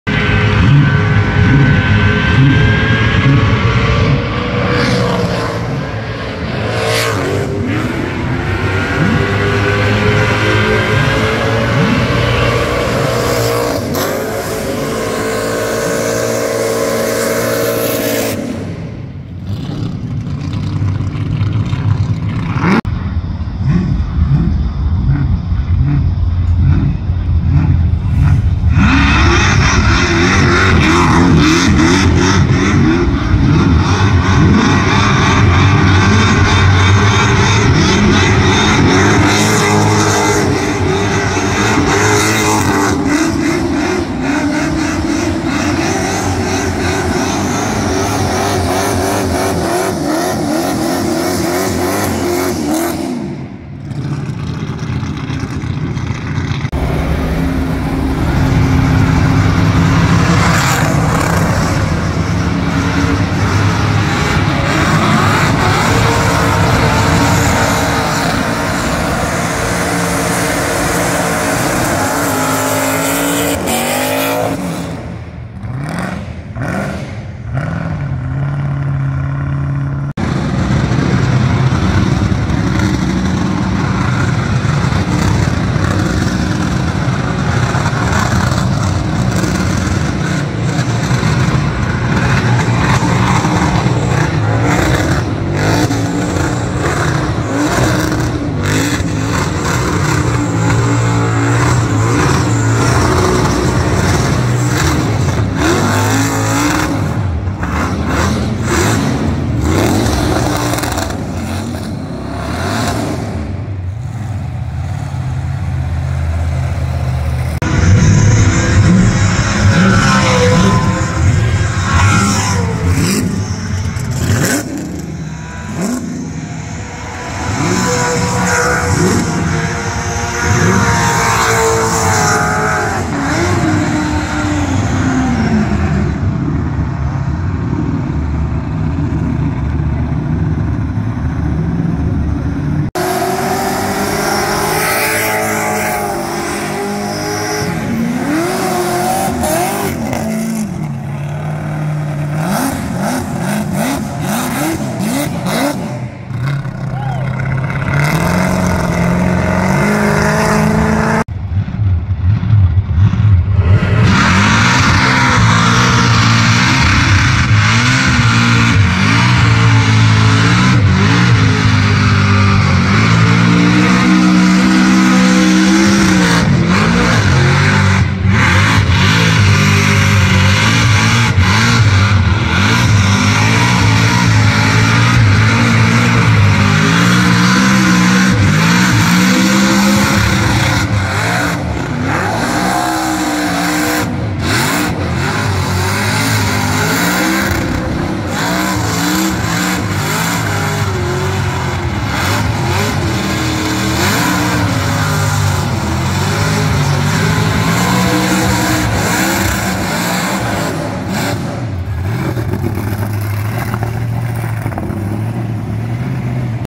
Day 3: Dream Team burnout display at Powercruise 95 Sydney
The dream team doing a burnout exhibition on the 3rd day at Sydney Motorsport Park for Powercruise 95. They will be heading to America soon to show them how aussies do burnouts